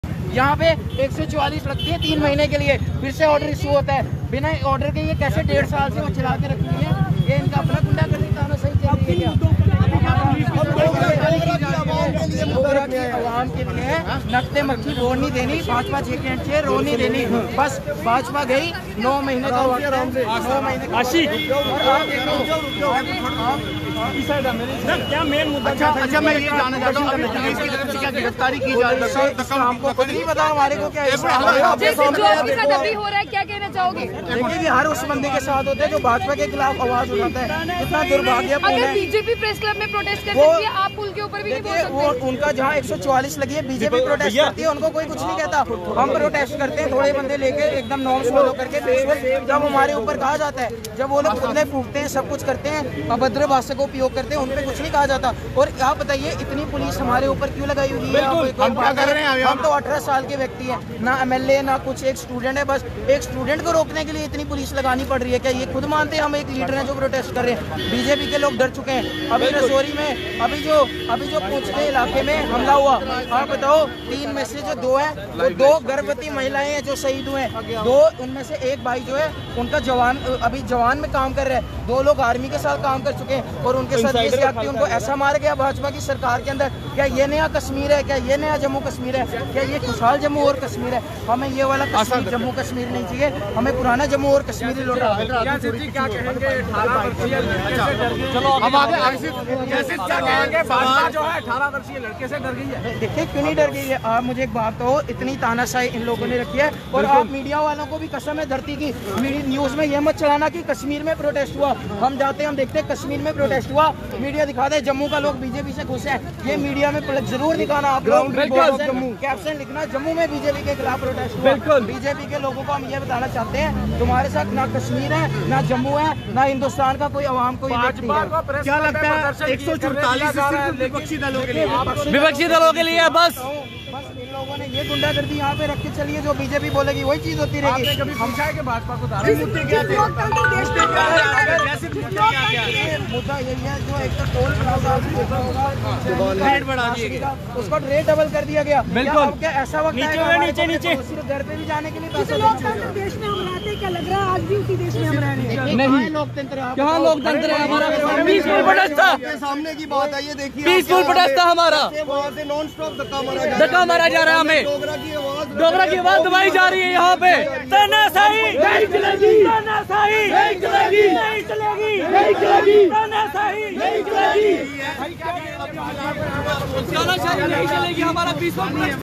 JLKMEDIA Congress Senior Leader Raman Bhalla के बेटे Jai Sidh Bhalla को
jlkmedia Congress Senior Leader Raman Bhalla के बेटे Jai Sidh Bhalla को Maharaja Hari Singh statue पर धरना प्रदर्शन करने से JK Police द्वारा रोका गया।